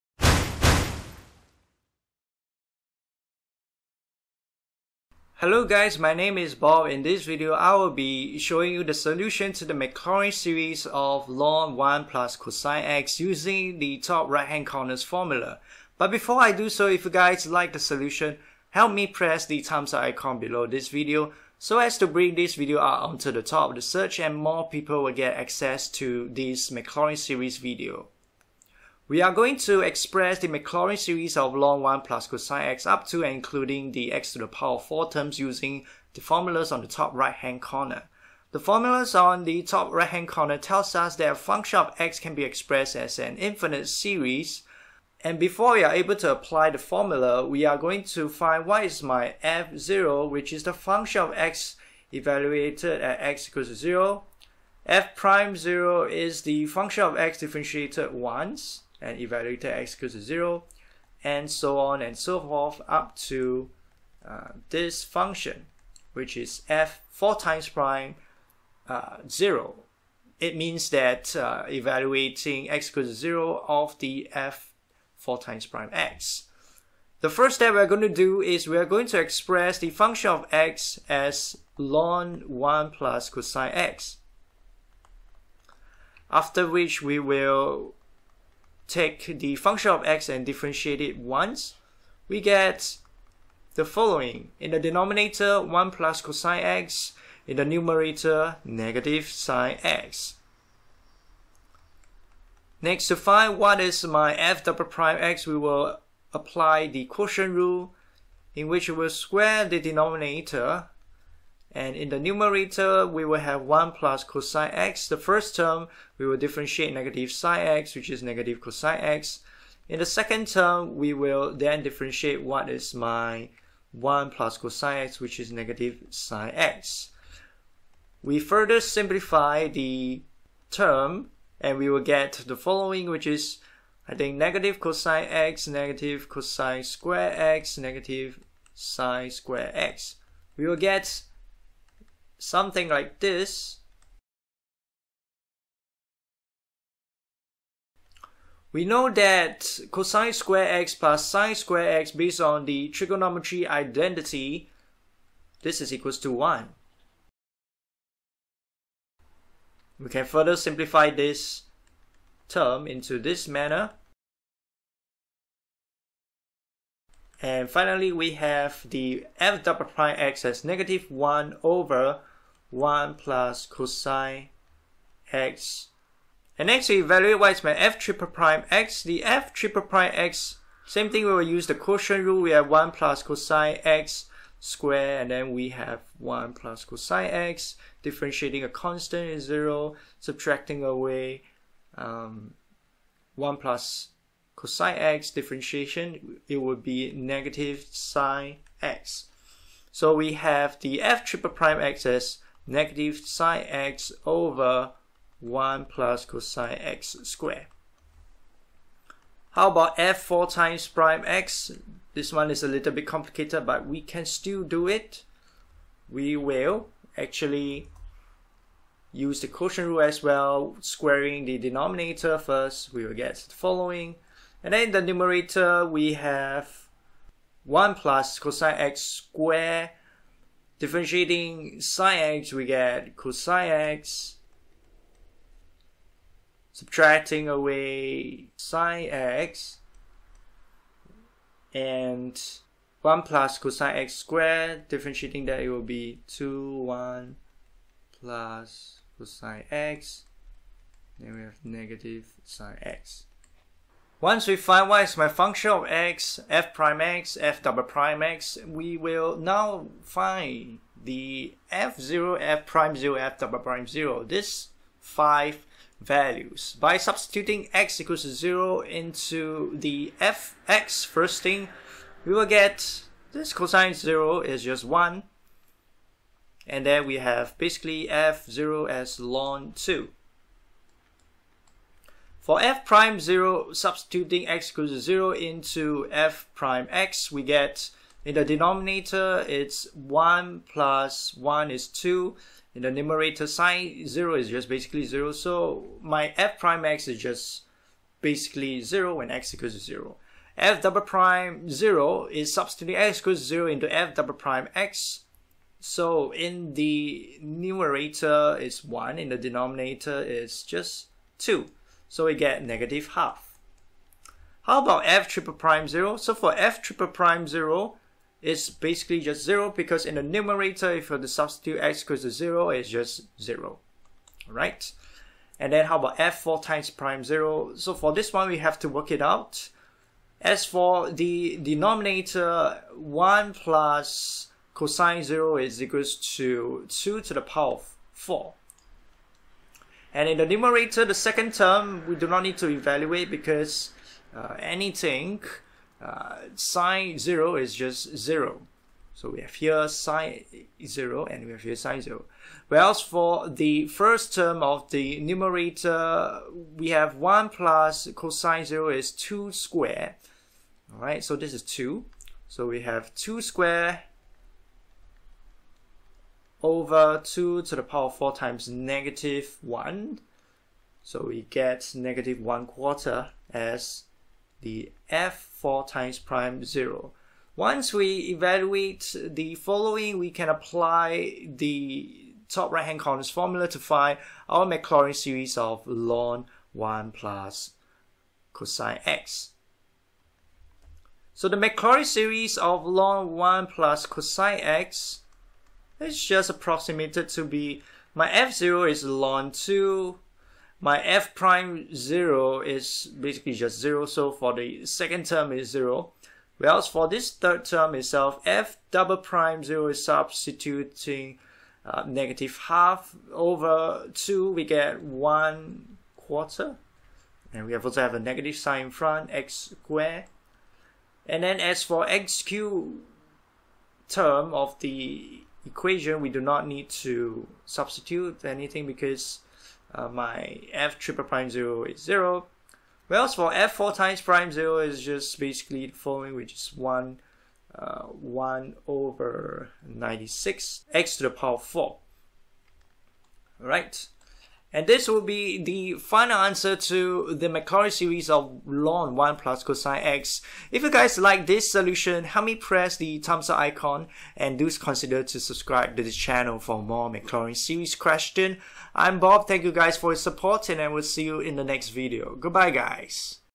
Maclaurin series of ln(1+cosx)
Maclaurin series of ln(1+cosx) (up to x^4 term) - this video teaches us how to perform the Maclaurin series of ln(1+cosx) (up to x^4 term)